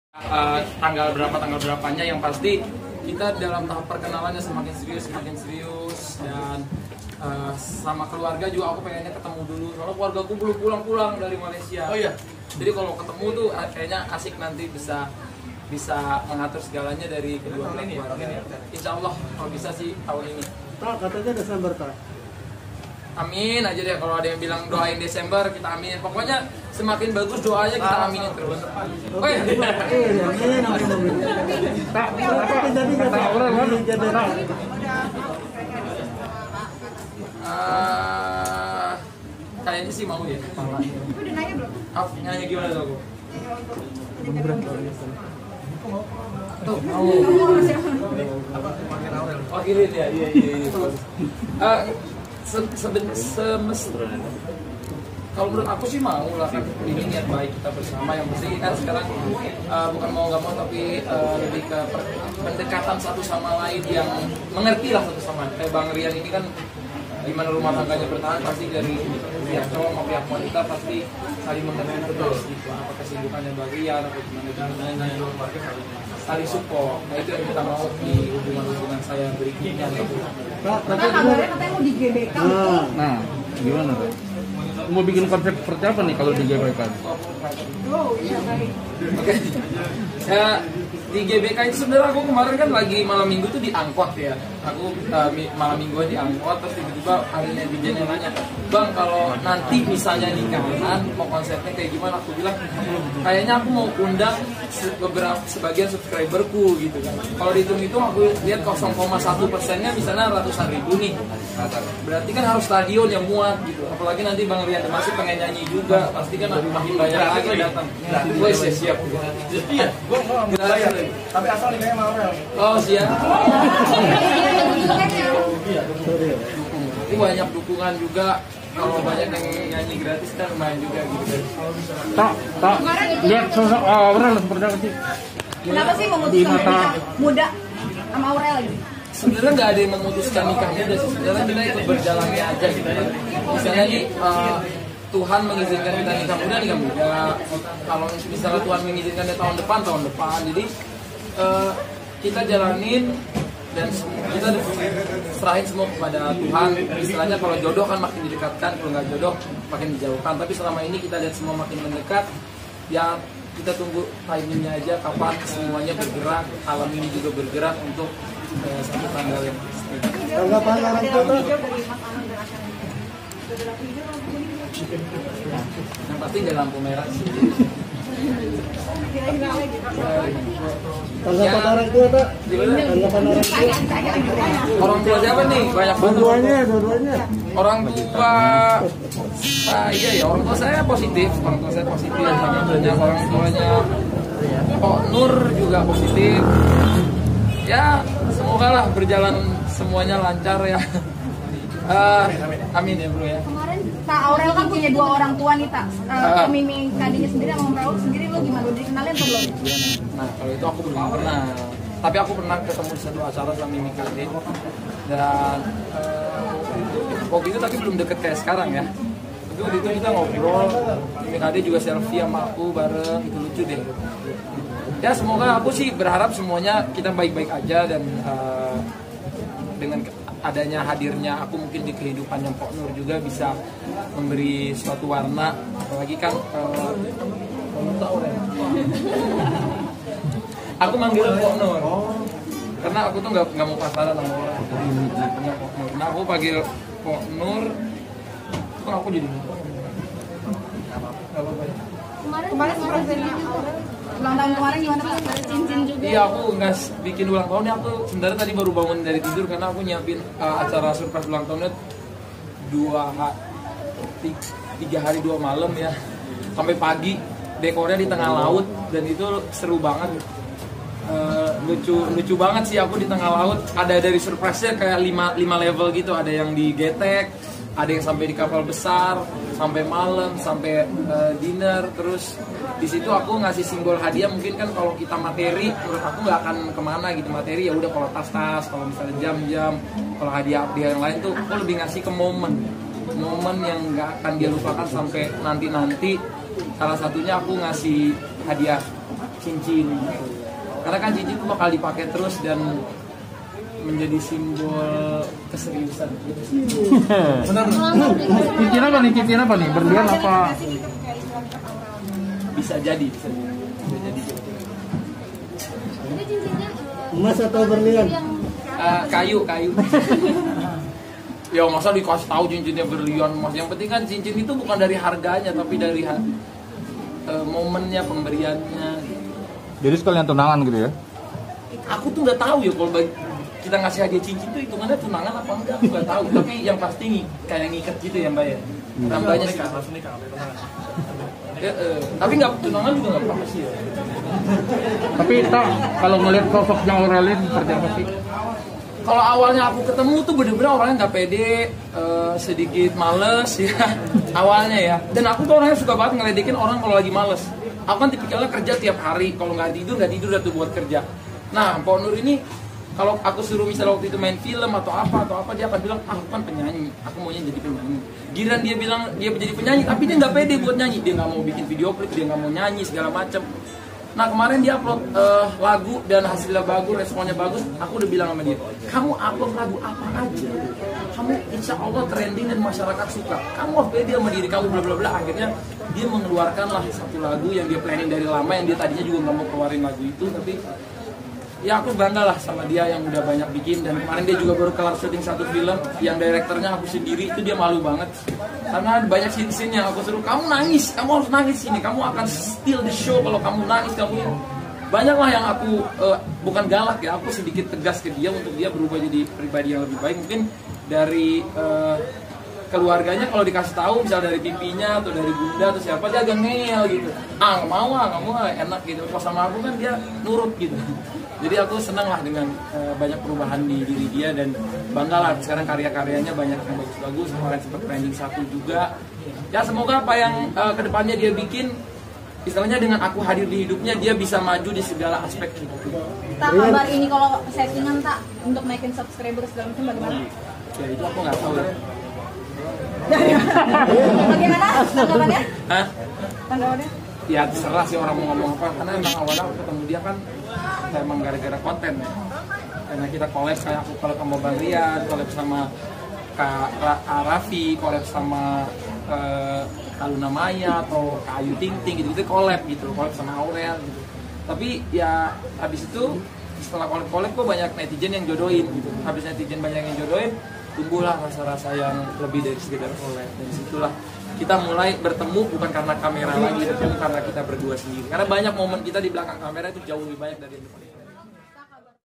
Jelang Nikah, ATTA HALILINTAR Melihat Banyak Perubahan pada AUREL HERMANSYAH
Atta Halilintar melihat banyak perubahan positif pada diri kekasihnya, Aurel Hermansyah. Atta pun semakin yakin untuk menjadikan Aurel sebagai pendamping hidup.